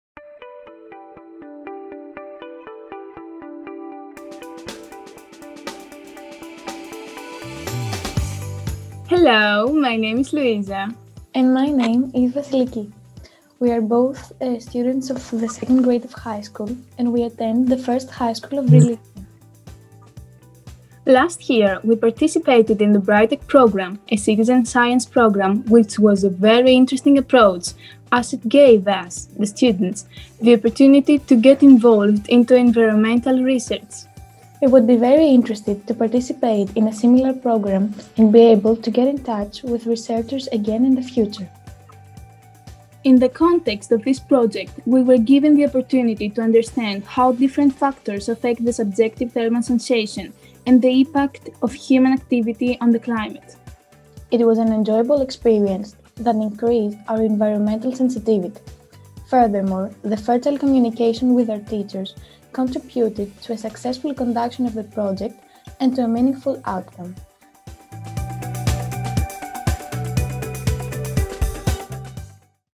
BRITEC IO3 3.1.1
The exemplary citizen science pedagogical practices and “science pills” videos developed under this output feed into the Citizen Science Toolkit; complemented with additional tools and guidelines, they may serve as guidance and inspiration for teachers and researchers from throughout Europe to bring research into classrooms.
Louiza Smyrni and Vassiliki Rizioti, 1st grade students in the 1st Vrilissia Upper High School in Athens, present their experience from participating in a Citizen Science project in Greece.